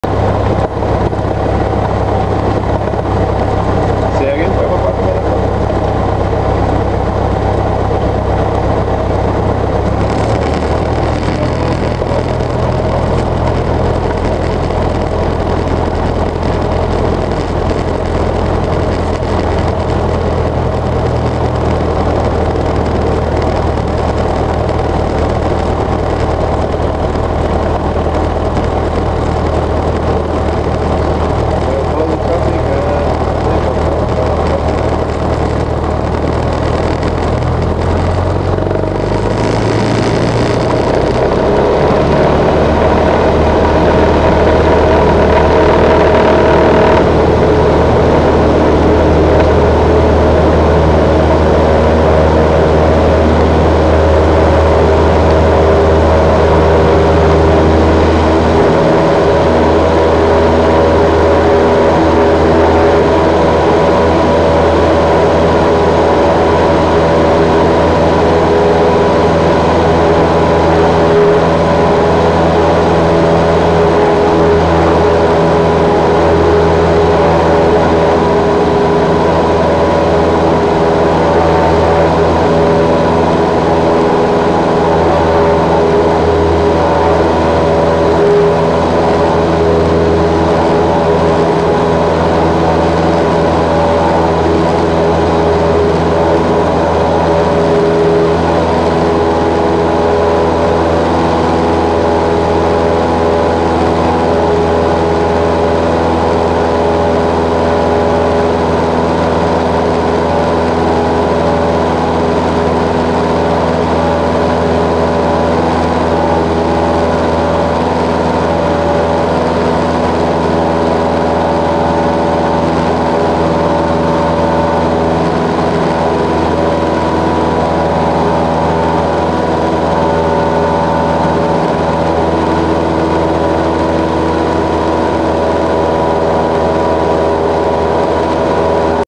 take off at ktmb first SOLO !!!
my take off at ktmb doing my first solo!